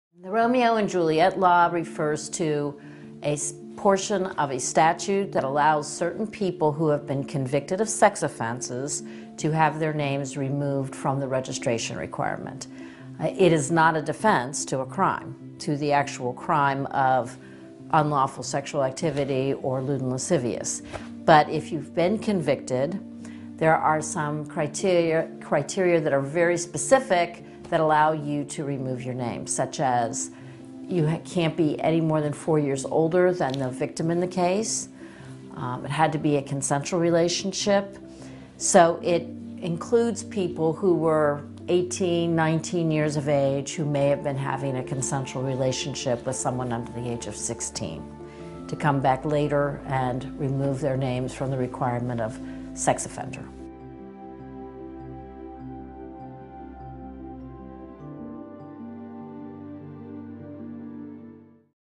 What is the Romeo and Juliet Law?
The Romeo and Juliet Law refers to a portion of a statute that allows certain people who have been convicted of sex offenses to have their names removed from the registration requirement.😳